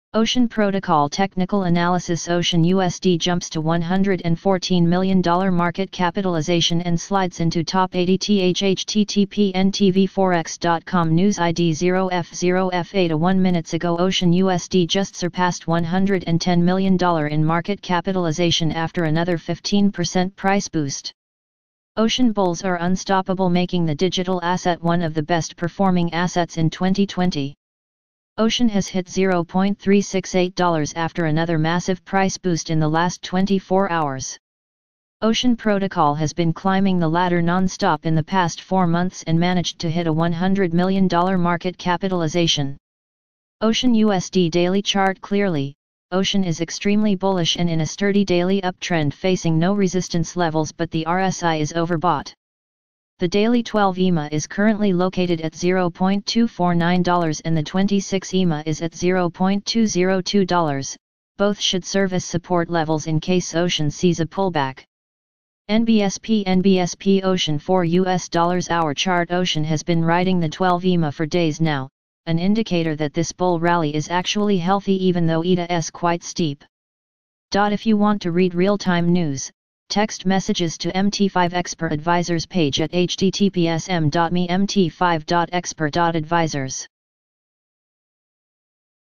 Ocean Protocol Technical Analysis: OCEAN/USD jumps to $114 million market capitalization and s...
Ocean Protocol Technical Analysis: OCEAN/USD jumps to $114 million market capitalization and slides into top 80th | 1 minutes ago
OCEAN/USD just surpassed $110 million in market capitalization after another 15% price boost.
OCEAN bulls are unstoppable making the digital asset one of the best-performing assets in 2020.
OCEAN has hit $0.368 after another massive price boost in the last 24 hours. Ocean Protocol has been climbing the ladder non-stop in the past four months and managed to hit a $100 million market capitalization.
OCEAN/USD daily chart
Clearly, OCEAN is extremely bullish and in a sturdy daily uptrend facing no resistance levels but the RSI is overbought. The daily 12-EMA is currently located at $0.249 and the 26-EMA is at $0.202, both should...